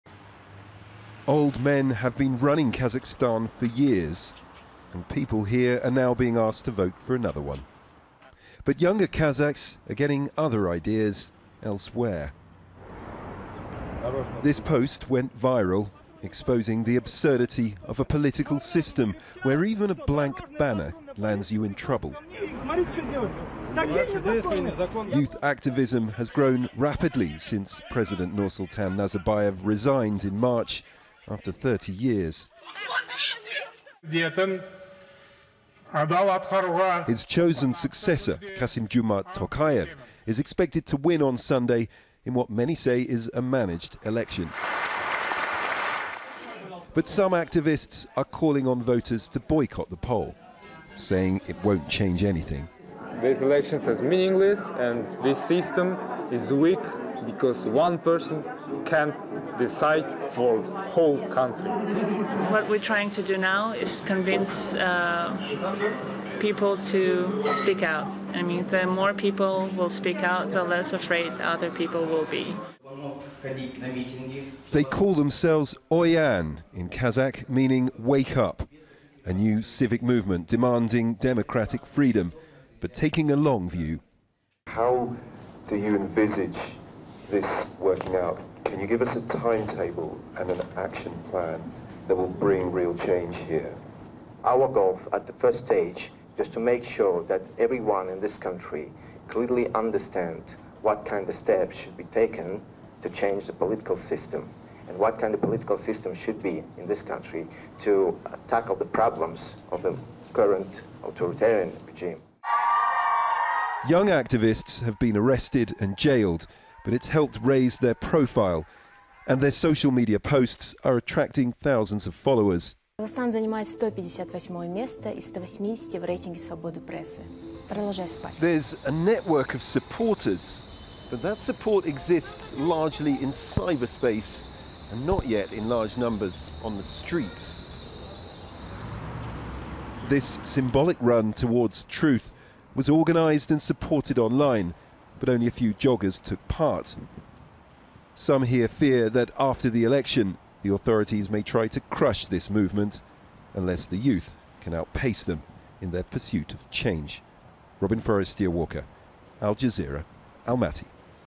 Kazakhstan Elections: Vote for new president on Sunday
Kazakhs are going to the polls on Sunday to choose a new president, for the first time in almost 30 years.
Interim President Kassym-Jomart Tokayev has been handpicked by the country's former leader, Nursultan Nazarbayev.
Al Jazeera's Robin Forestier-Walker reports from Almaty.